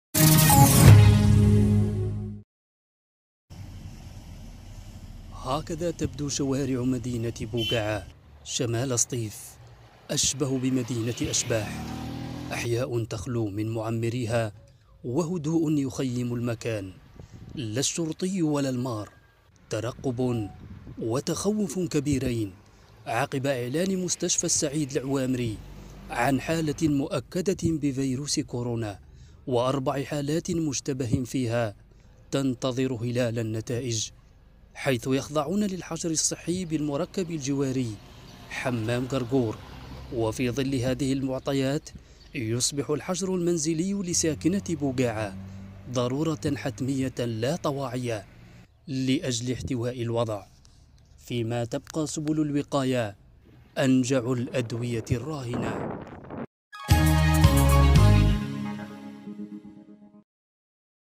وطنية نيوز| شوارع بوقاعة تحن لساكنيها بعد تسجيل إصابة بـ"كورونا"
شوارع بوقاعة تحن لساكنيها بعد تسجيل إصابة بـ"كورونا"